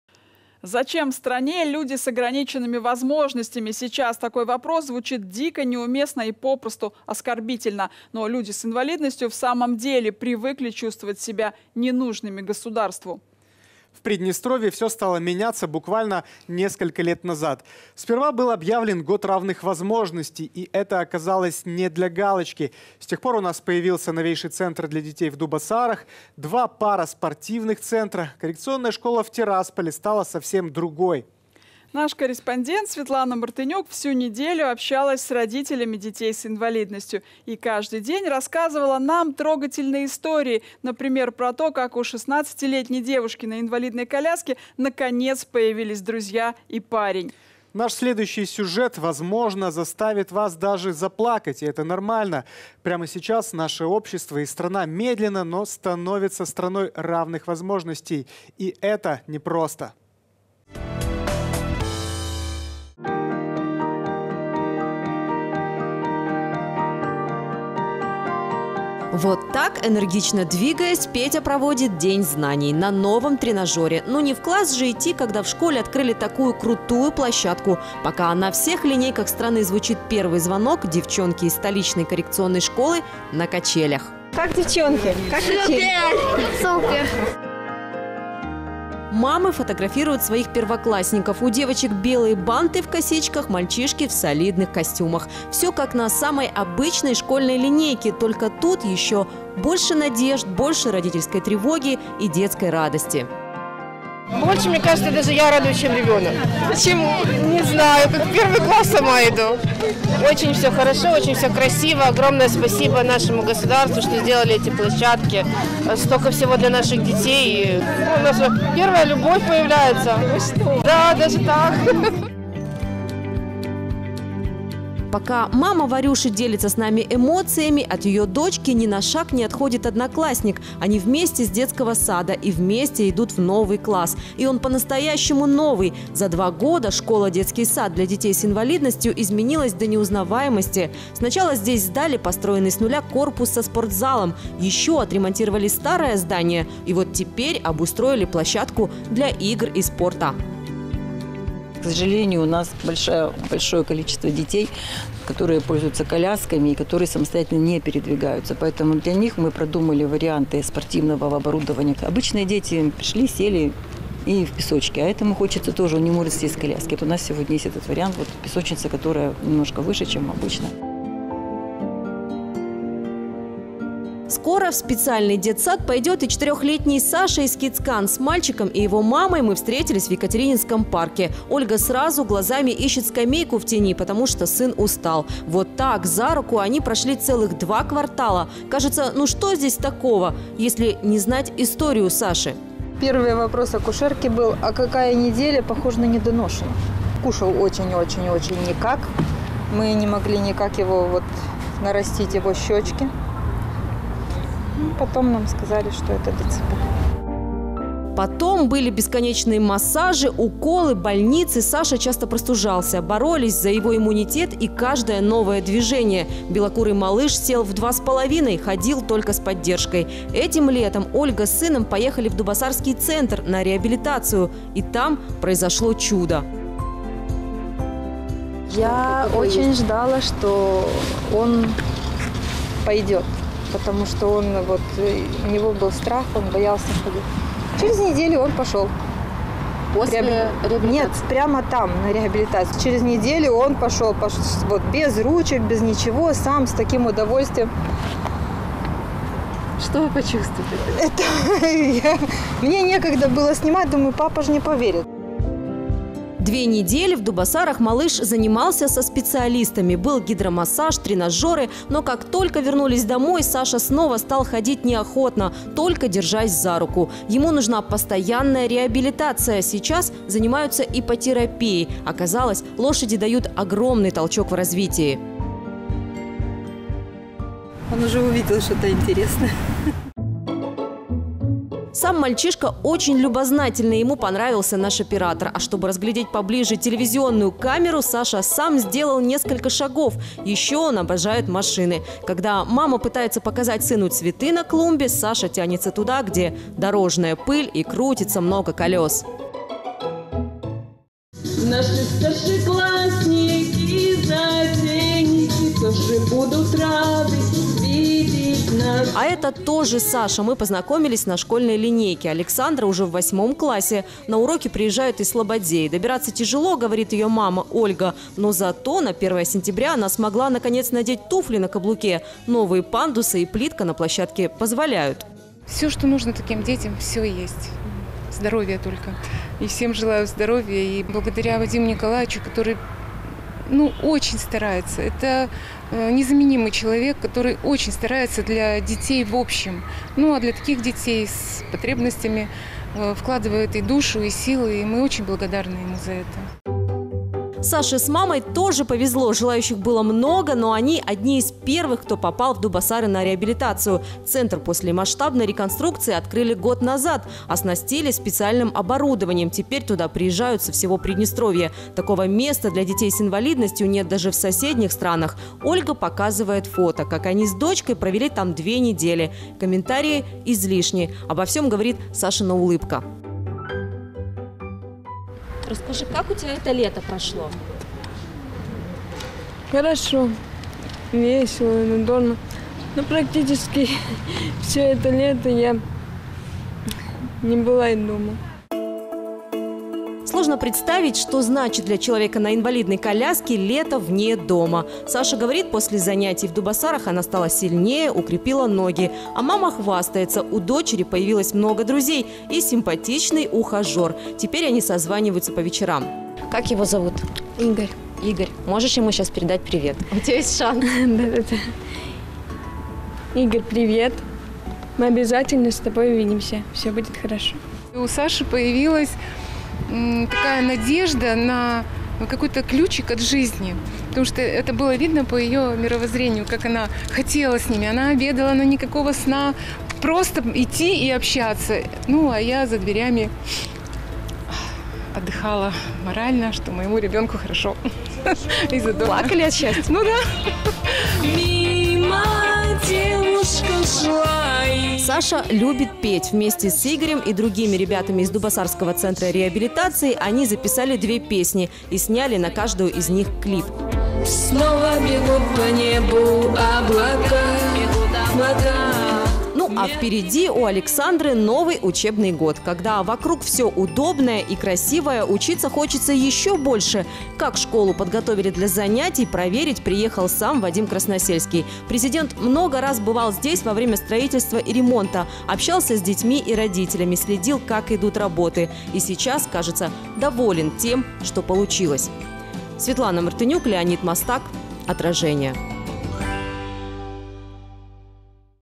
Страна равных
Зачем стране люди с ограниченными возможностями. Сейчас такой вопрос звучит дико, неуместно и попросту оскорбительно. Но люди с инвалидностью в самом деле привыкли чувствовать себя не нужными государству. В Приднестровье всё стало меняться буквально несколько лет назад. Сперва был объявлен год равных возможностей. И это оказалось не для галочки. С тех пор у нас появился новейший центр для детей в Дубоссарах, два пара-спортивных центра, коррекционная школа в Тирасполе стала совсем другой. Наш следующий сюжет, возможно, заставит вас даже заплакать, и это нормально. Прямо сейчас наше общество и страна медленно, но становится страной равных возможностей. И это непросто.